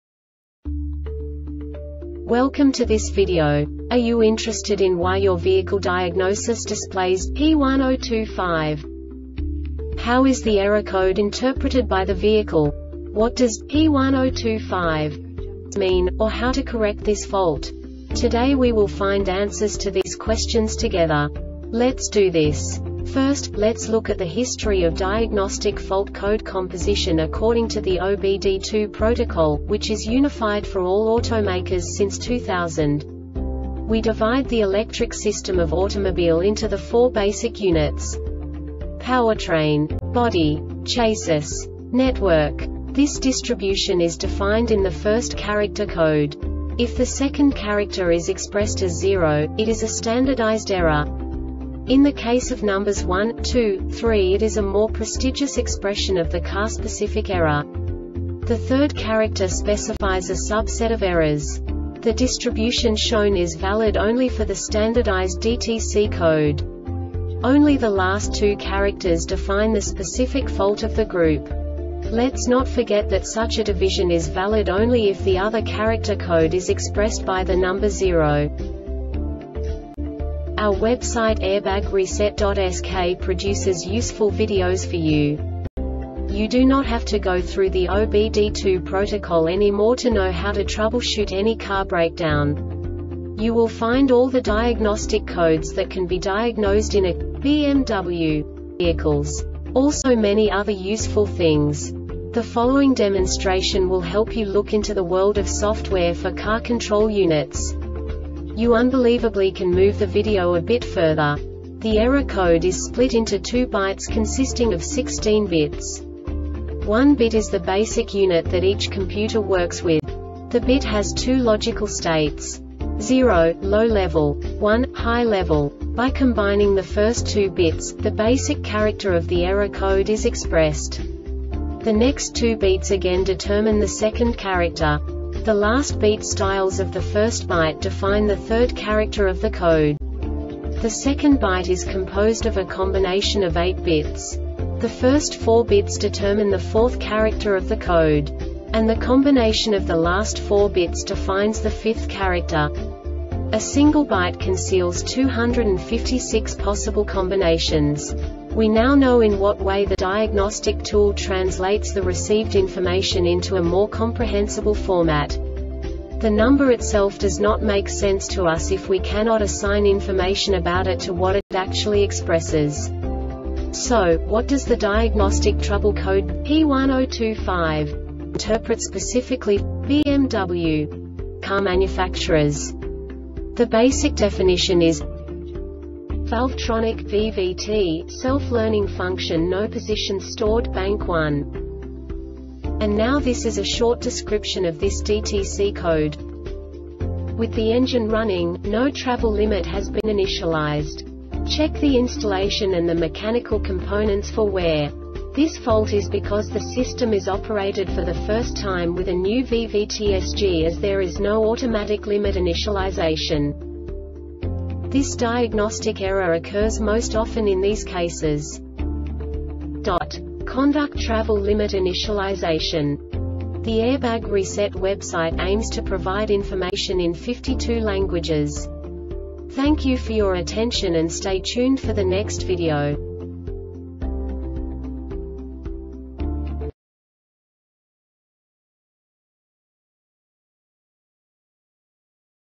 DTC BMW P1025 Short Explanation
Contents:
0:21 Basic DTC analysis according to OBD2 protocol standard.
1:48 Insight into programming
2:58 A diagnostically understandable description of the Diagnostic Trouble Code
3:05 Basic error definition

Make:
BMW
Code:
P1025
Definition:
Valvetronic (VVT) Self-Learning Function No Positions Stored (Bank 1)
Description:
With the engine running, no travel limit has been initialized. Check the installation and the mechanical components for wear. This fault is because the system is operated for the first time with a new VVT-SG as there is no automatic limit initialization.
Cause:
Conduct travel limit initialization.